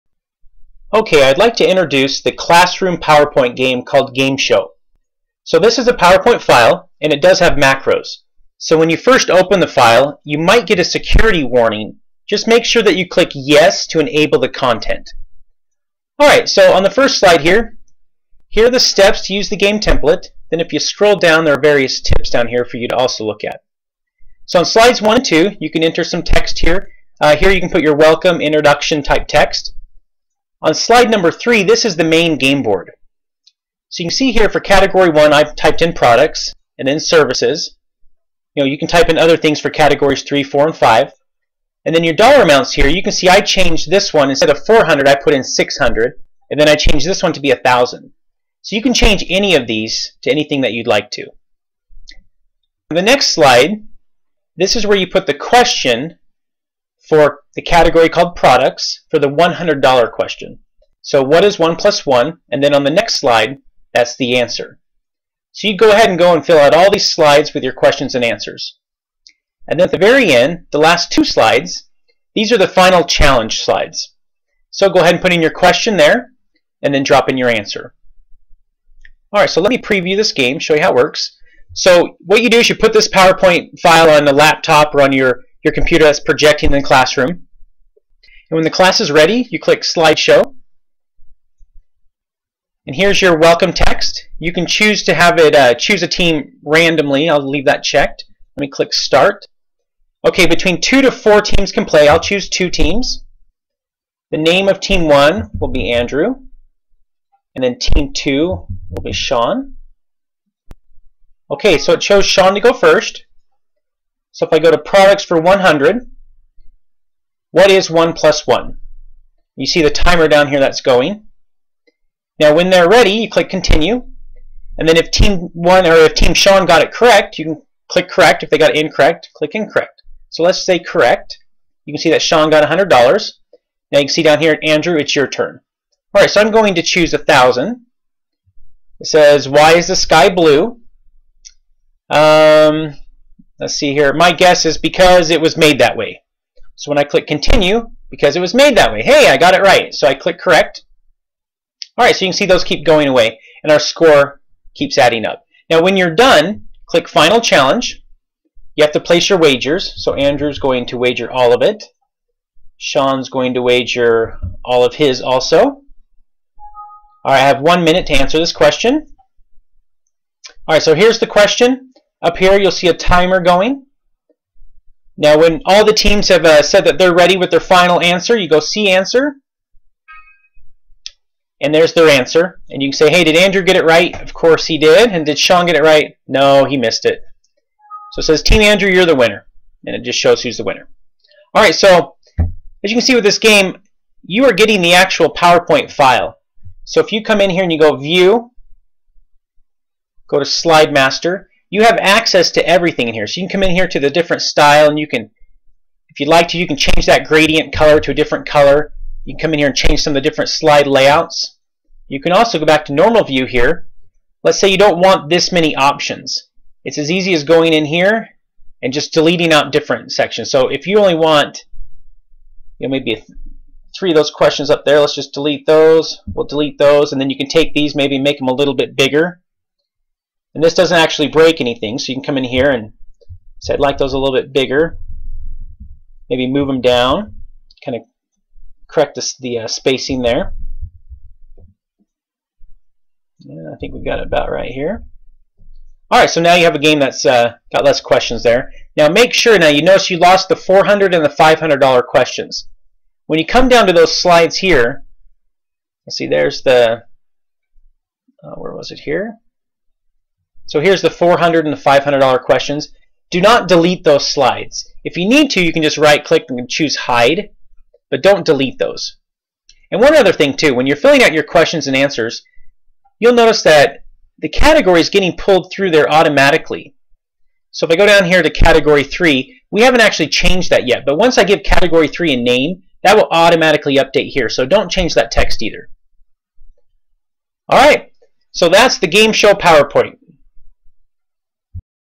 PowerPoint GameShow Template Tutorial
This is a video showing the GameShow PowerPoint classroom game.